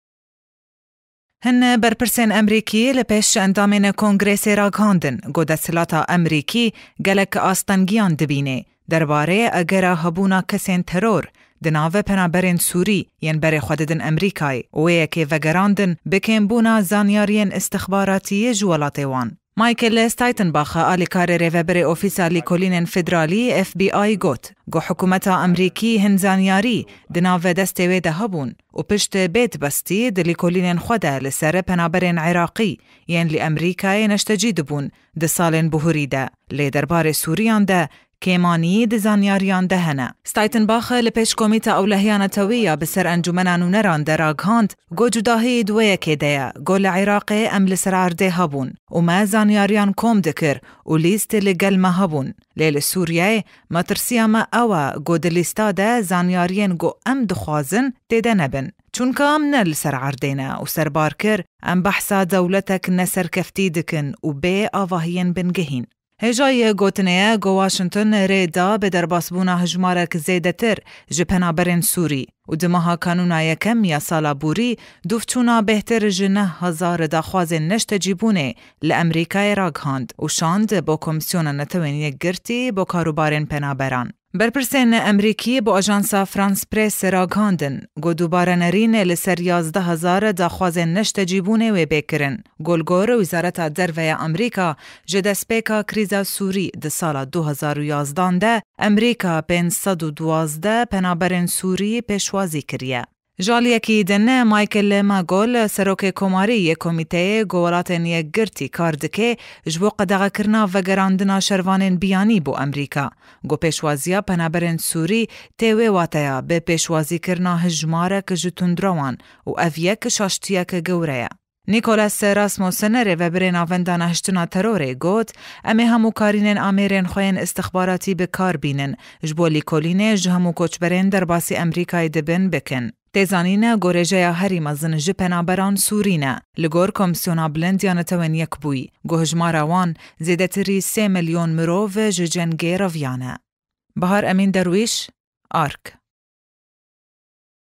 Metirsiyek Emrîkî bi hebûna tendrewan nava neşteciyên Sûrî li Emerîka
تخوف أمريكي من وجود ارهابيين بين المهاجرين إلى أراضيها